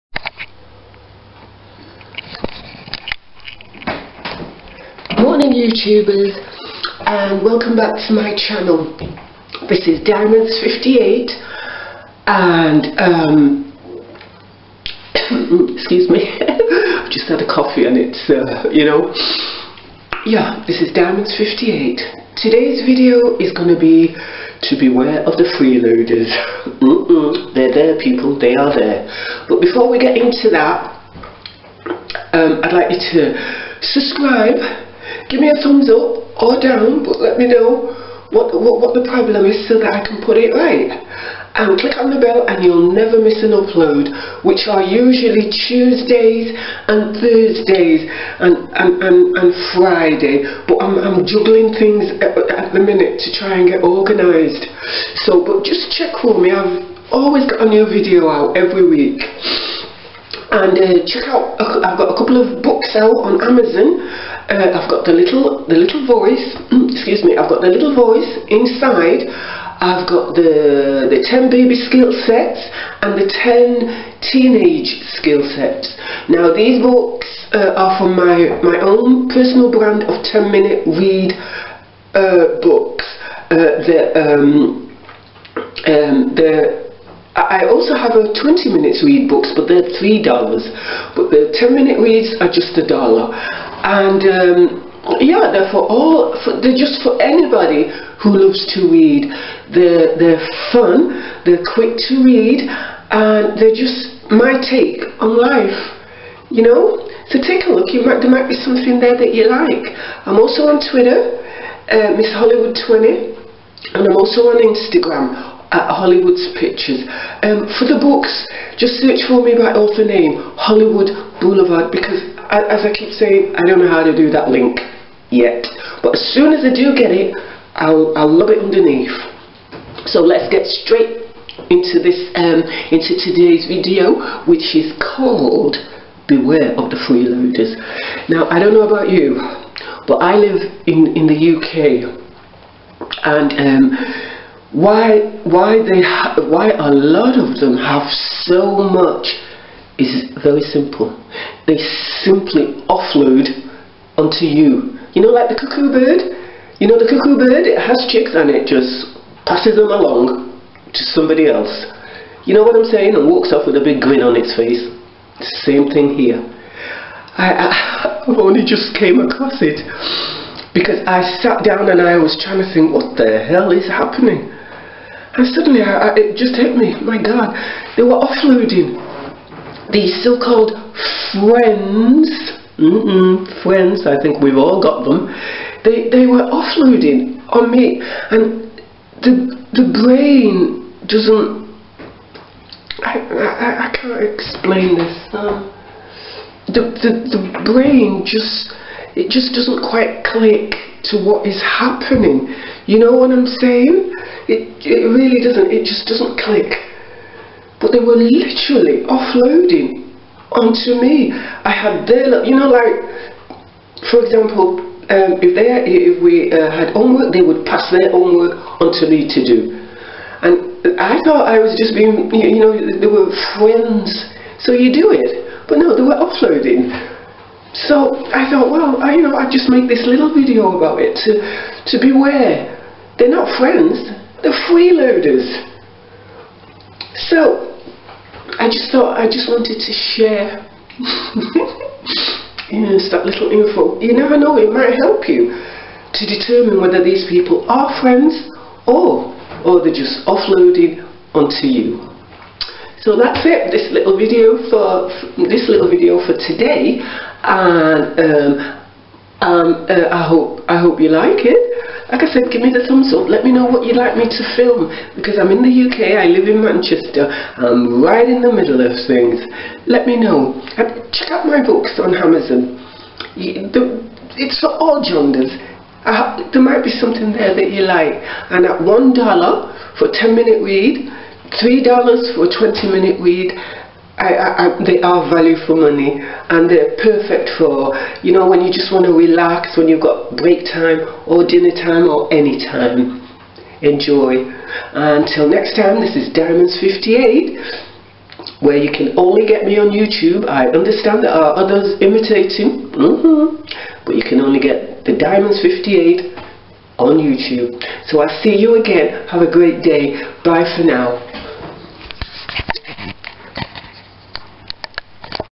Beware, Freeloaders
Wanted to share with you one of my life's lessons that I learnt years ago, it`s were it belongs in the past.  Uploads Tuesday, Thurs may be other days too, look out for me.